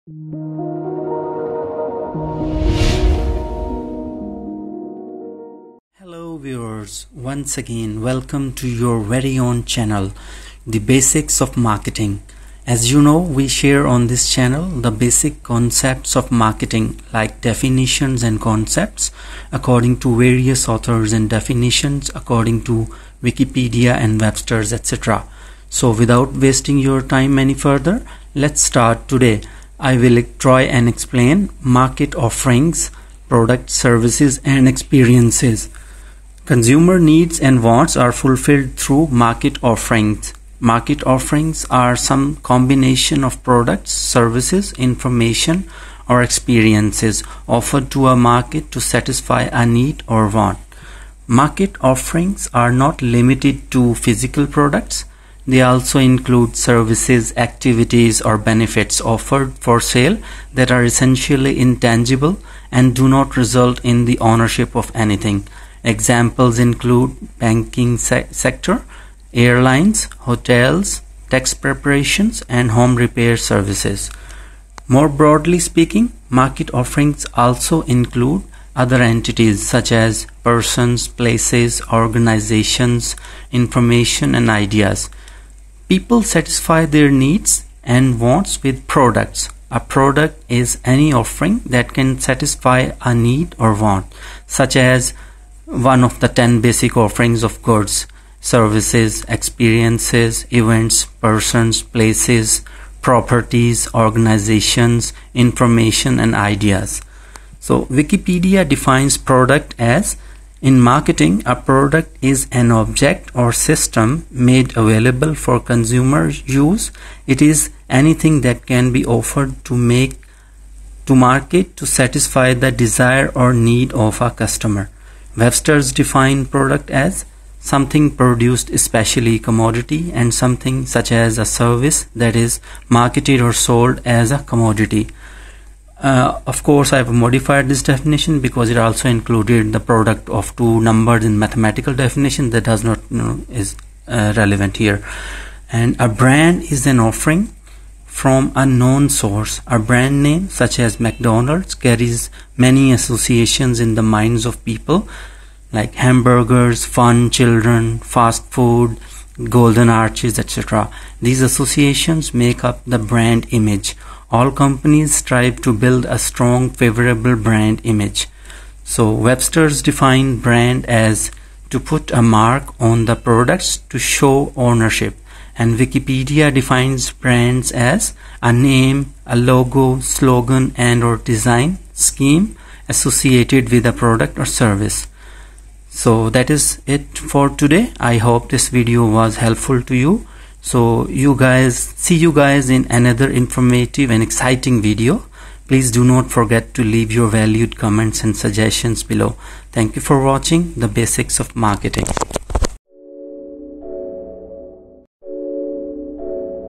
Market Offerings - Products, Services & Experiences.
In this video I have tried to explain the idea of Market Offerings that include Products, services & Experiences. I have tried to explain Brand name as well at the end of video. This video is a good start point to understand marketing basic steps...